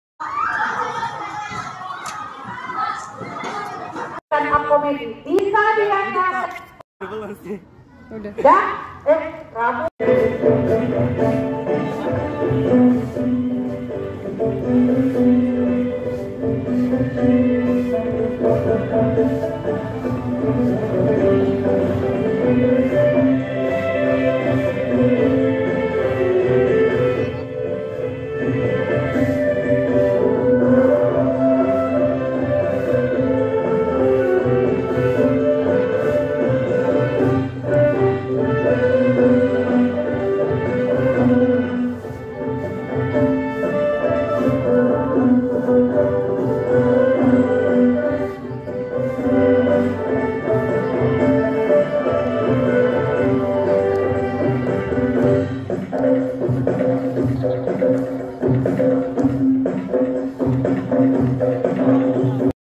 Tampilan Unjuk Bakat Siswa
Kegiatan yang di lakukan oleh sekolah untuk mengekspose kemampuan peserta didiknya dengan menampilkan unjuk bakat yaitu menari